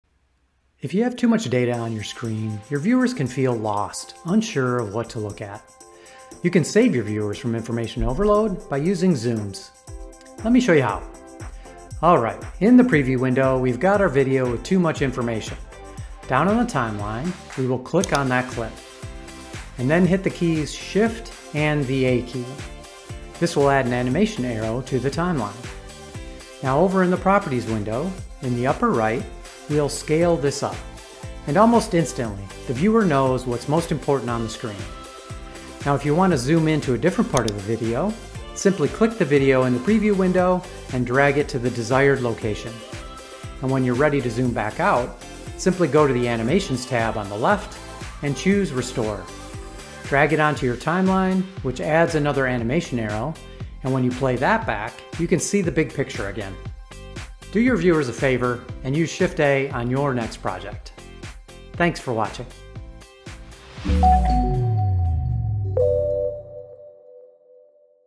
Easily View the Most Important Content with Camtasia's Zoom
When you have a lot displayed on your screen, or you have to squint to see the most important stuff, try zooming in. This can make your Camtasia videos easier to view.

Camtasia in 90 Seconds